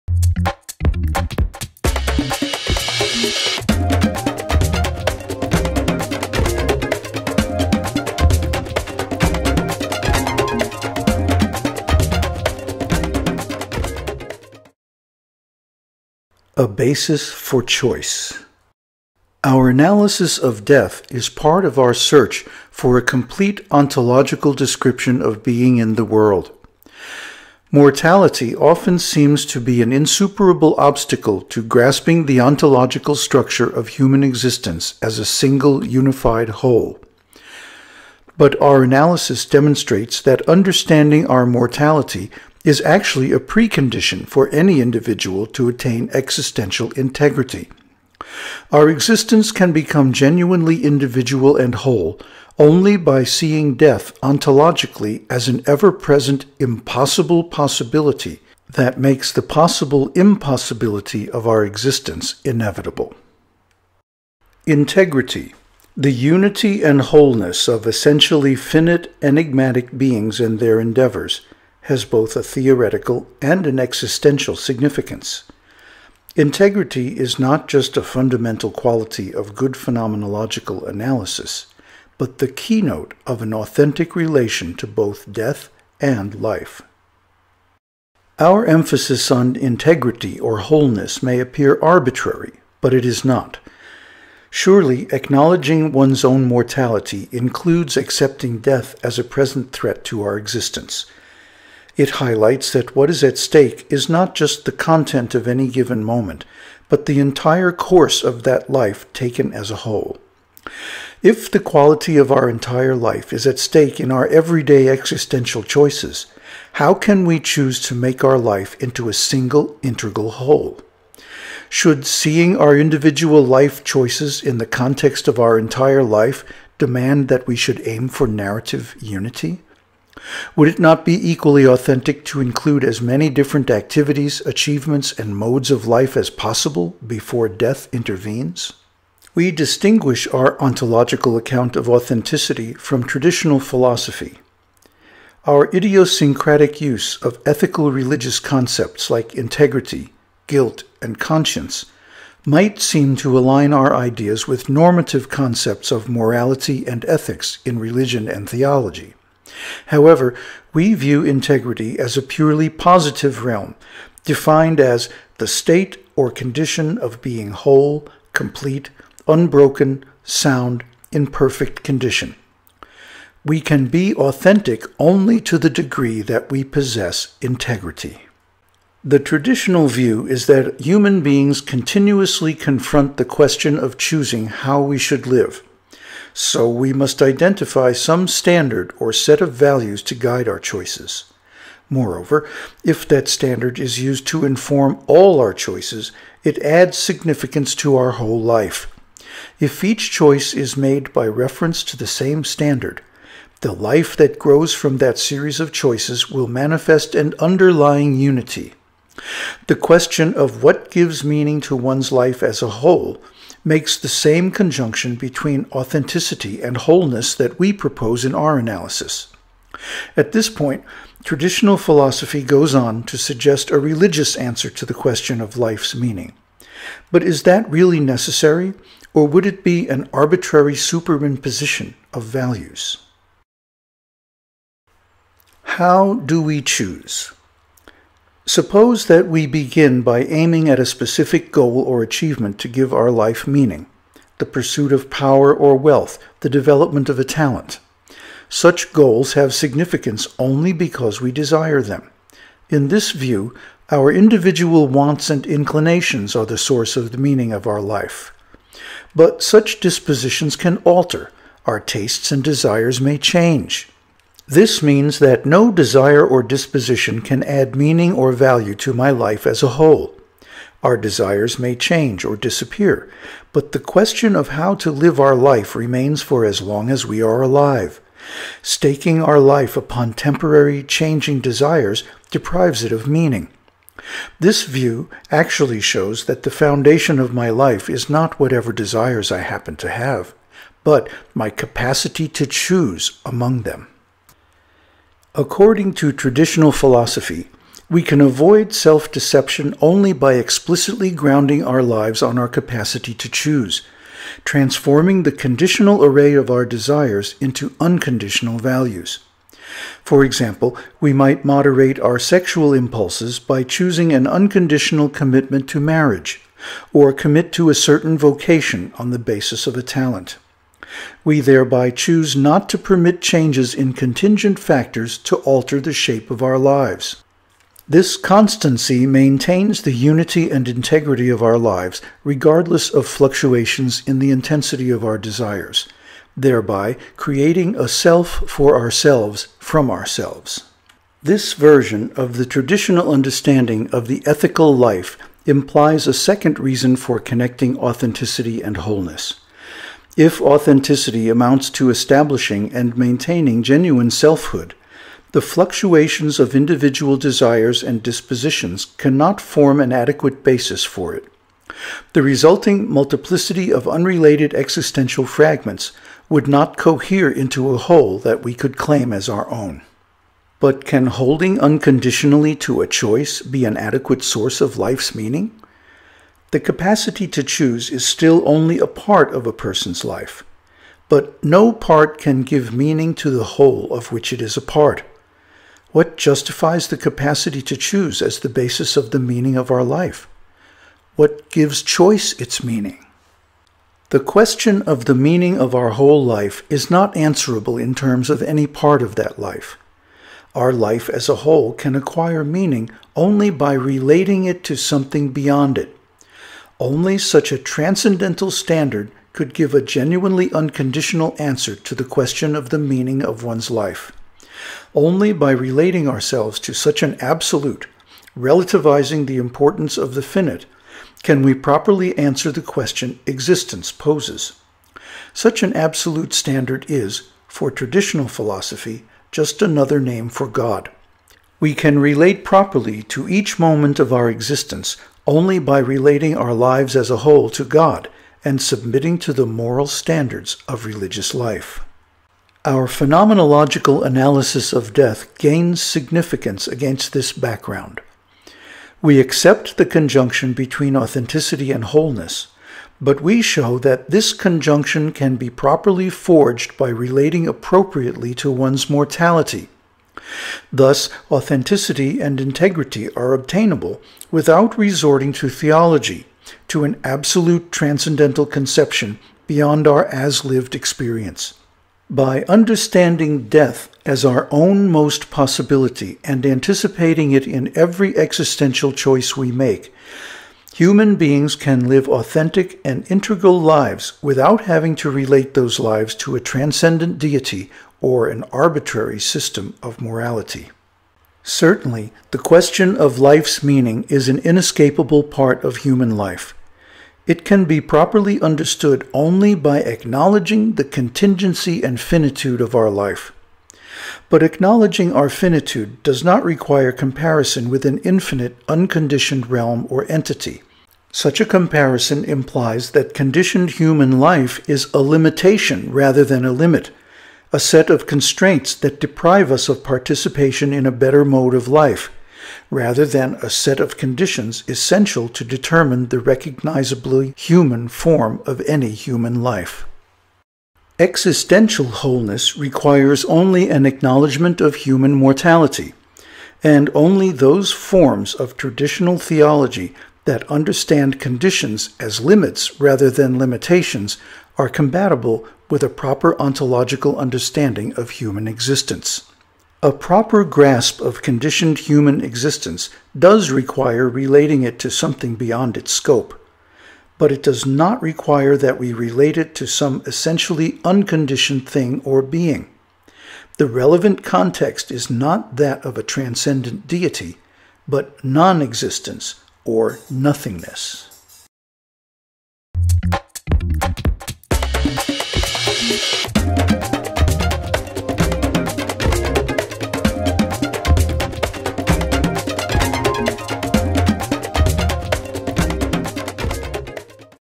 Being in the World 6: A Basis for Authentic Choice
Being in the World is a deep analysis of Heidegger's masterpiece Being and Time. It is intended as an introduction to phenomenological thinking, which is also the basis of the Buddha's teaching. It is divided into readings and study guides, which supply questions and exercises for you to get the most value from the material.